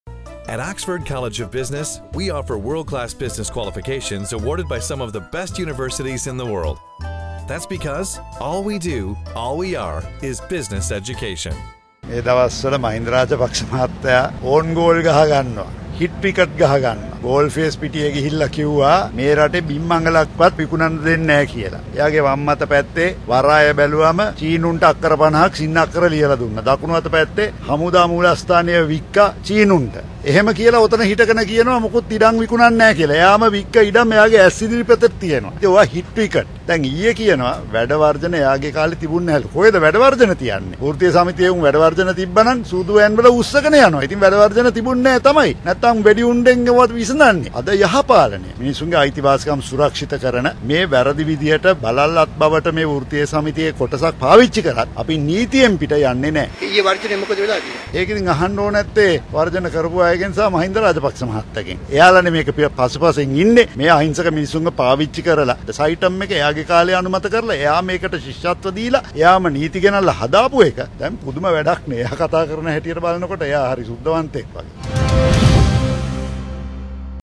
We won't go beyond the law - Kabir Hashim
මේ දවස්වල මහින්ද රාජපක්ෂ මහත්තයා හිට්විකට් ගහගන්නවා... කබීර් හෂීම්